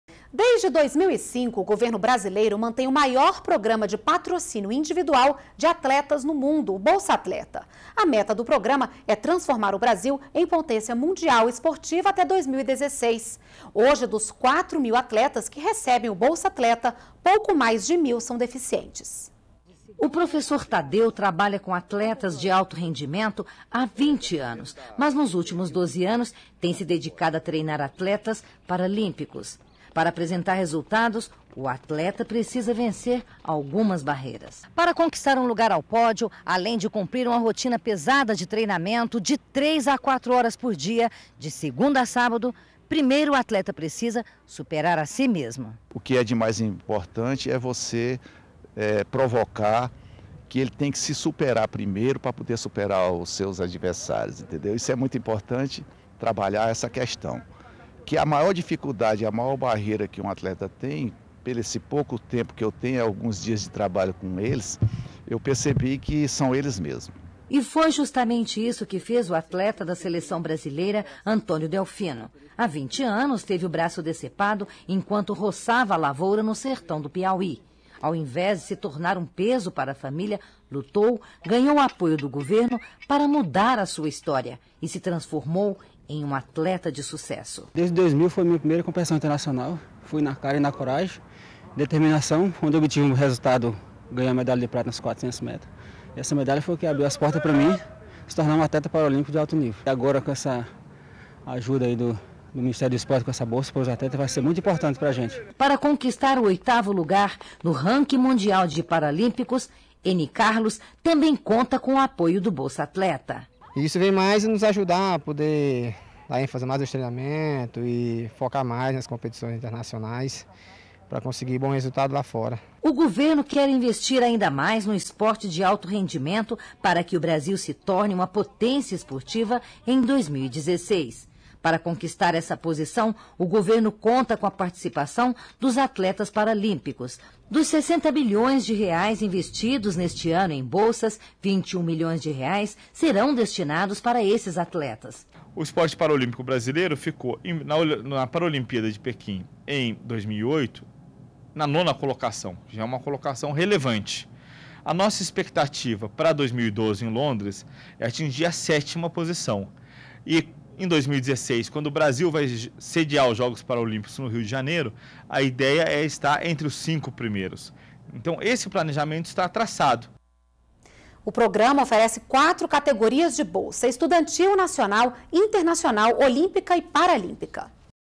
Governo quer investir para que Brasil seja uma potência em esporte de alto rendimento em 2016
NBR NOTÍCIAS - 12.04.12: Desde 2005, o governo brasileiro mantém o maior programa de patrocínio individual  de atletas no mundo, o Bolsa Atleta. A meta do programa é  transformar o Brasil em potência mundial esportiva, até 2016. Hoje, dos quatro mil atletas que recebem o Bolsa Atleta pouco mais de mil são deficientes. O programa oferece quatro categorias de bolsa: Estudantil, Nacional, Internacional, Olímpica e Paralímpica.